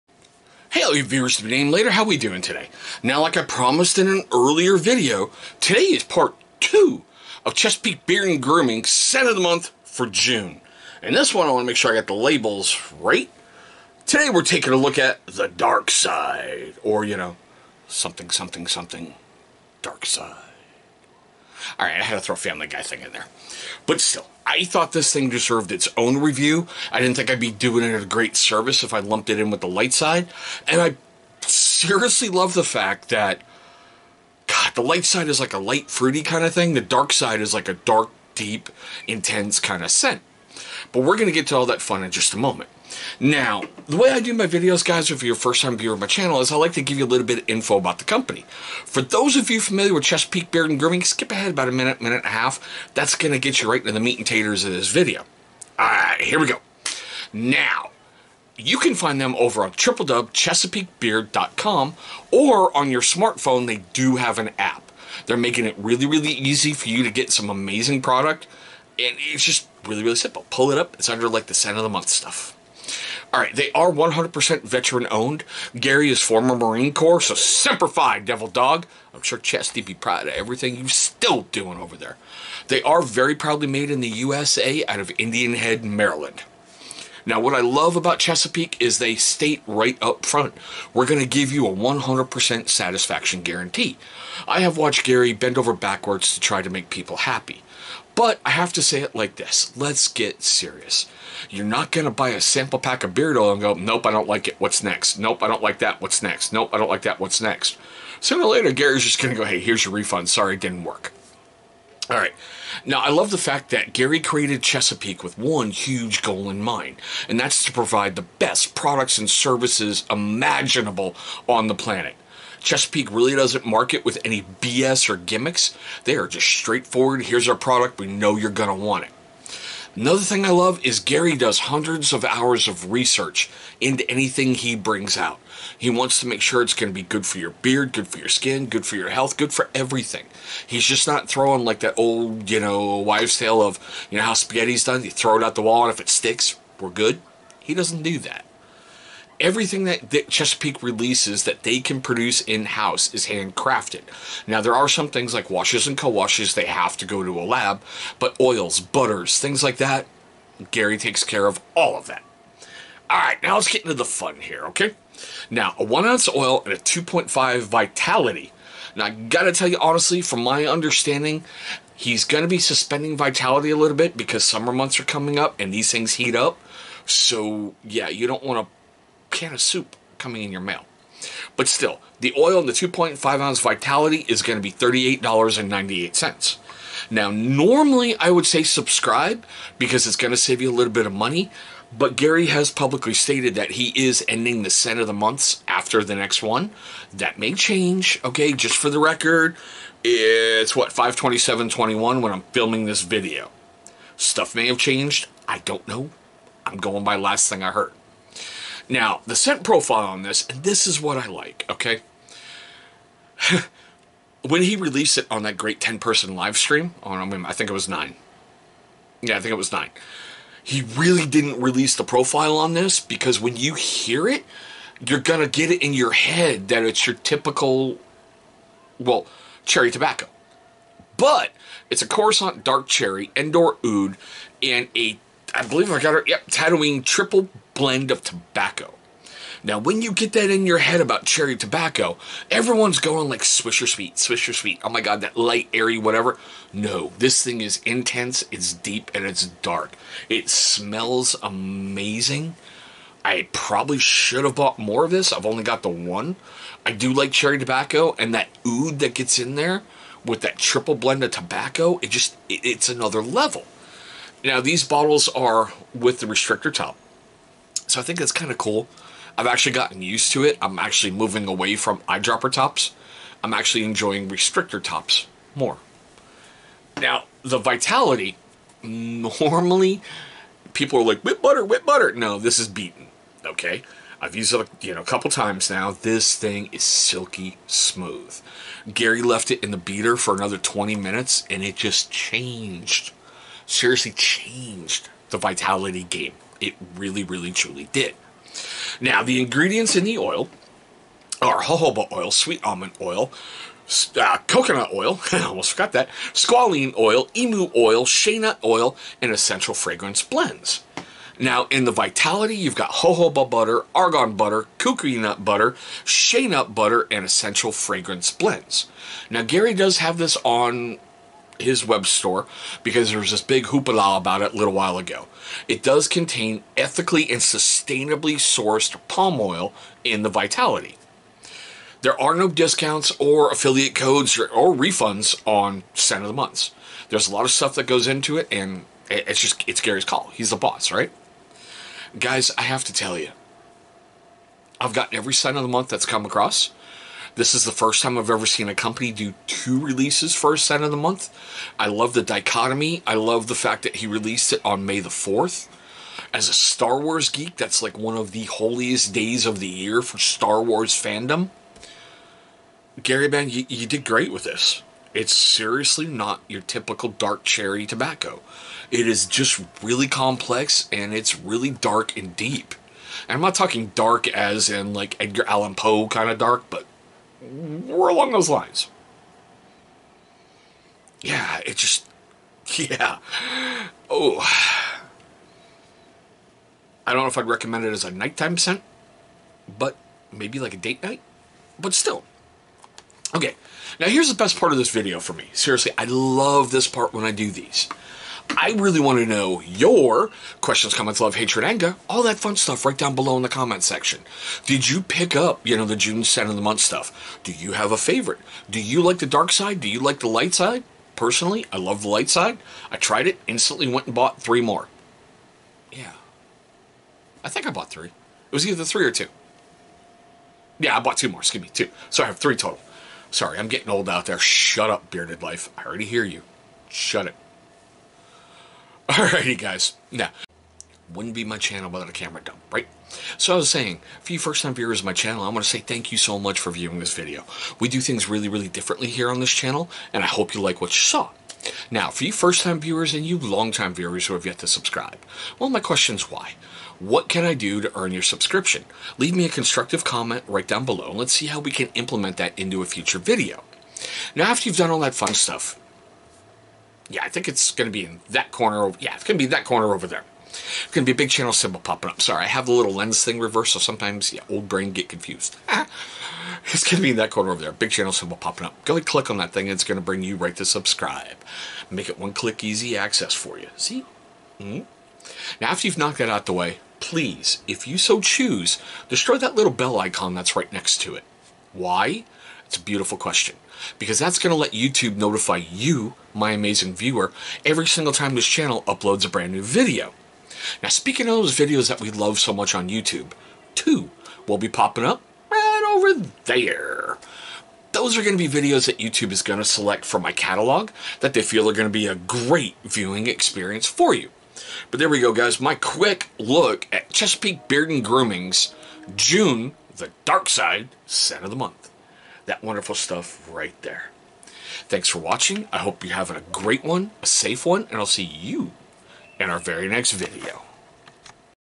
Chesapeake Beard & Grooming "The Dark Side" June 2021 SOTM
Chesapeake Beard & grooming is back on the channel. A little bit ago i promised i would cover this second part of June's scent of the month. The Dark Side is a deep, dark, dank awesome smelling choice for your beard. Even if you know the scent profile, it will blow your mind.

Gary brother, you have done a OUTSTANDING job once again.

You can buy this combo, or some other amazing products at